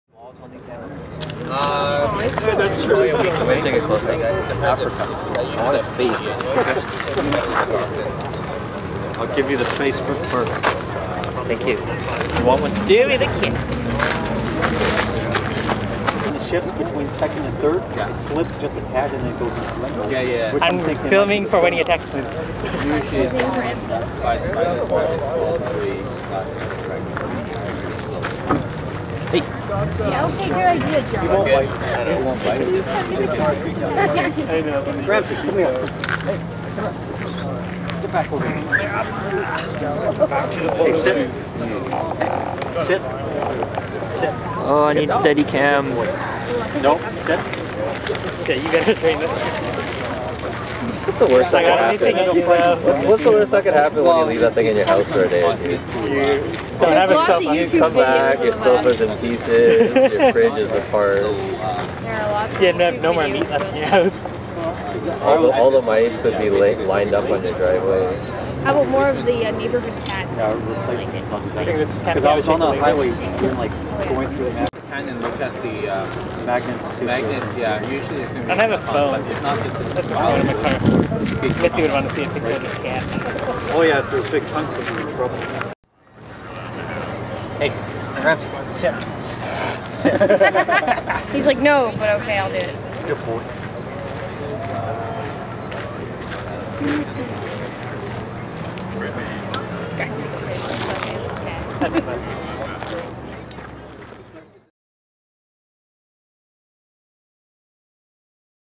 Serval (Leptailurus Serval) Tierboskat, "tiger-forest-cat"
Someone brought this feisty feline to a little car show. I felt sorry for the animal as it seemed stressed and just wanted to run away and/or hide. Everyone was crowding around it trying to snap a photo. Pretty sweet looking through.

Serval
Cat
Tiger-Forest-Cat
Tierboskat
Leptailurus Serval
Feisty Feline
Feline
Meow
Hissing